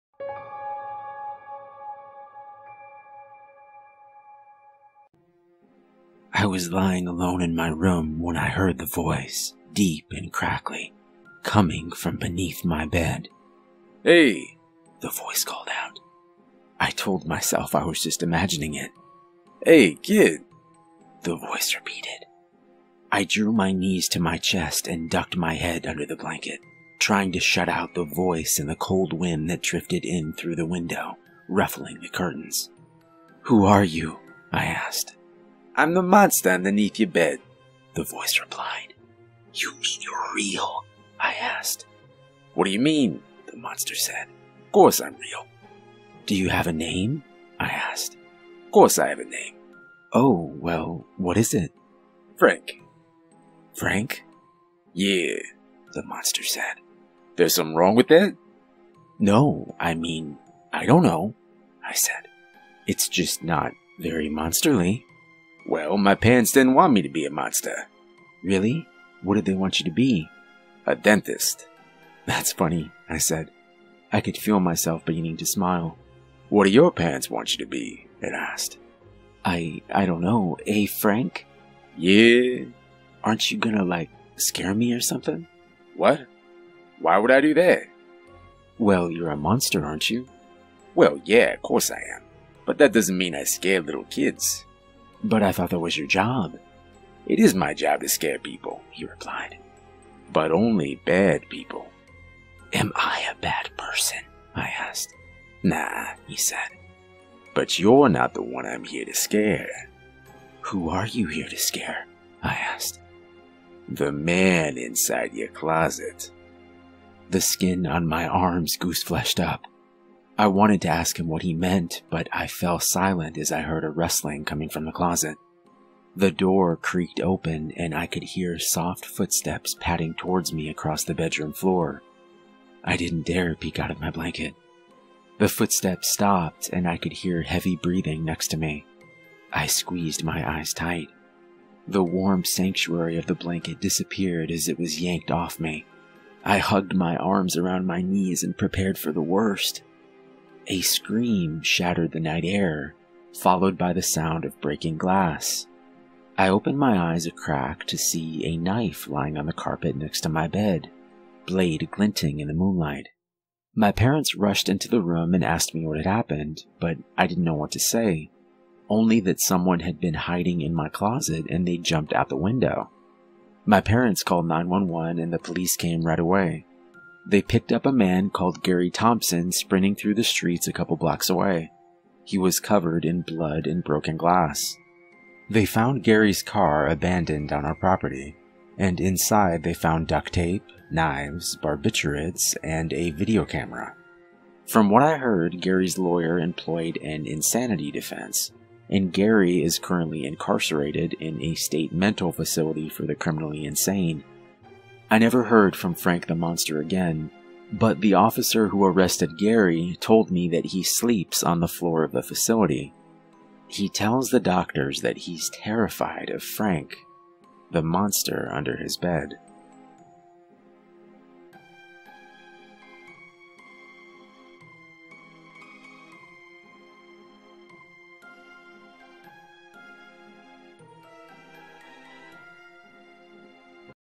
"The Monster Under Your Bed Isn't Always There To Scare You" | NoSleep Funny Story
I was lying alone in my room when I heard the voice, deep and crackly, coming from beneath my bed.

Narrated By: Lighthouse Horror

Special thanks to David for the amazing story and Duncan for letting me use his excellent artwork for this story! Thank you both!